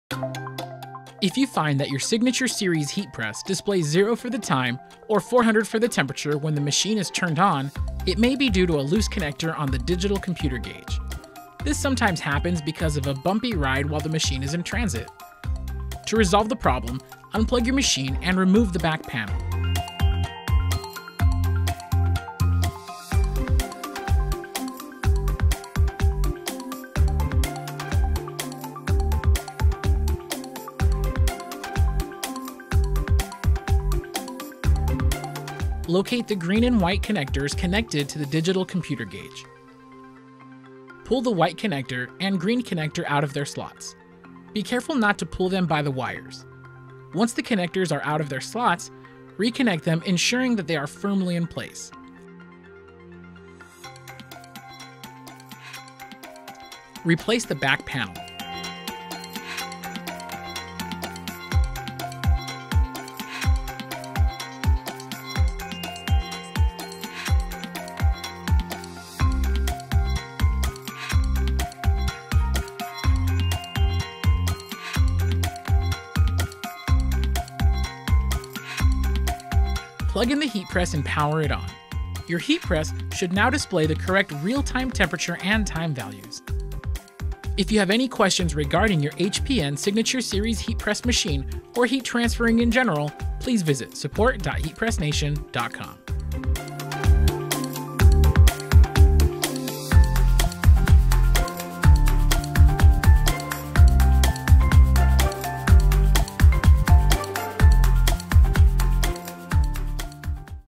MyExpert Troubleshooting - HPN Signature Series Non-Stop Beeping
This brief video shows how to fix the digital computer gauge to display the real time temperature and time settings properly.

Show us what you’ve created by using hashtag on Facebook or Twitter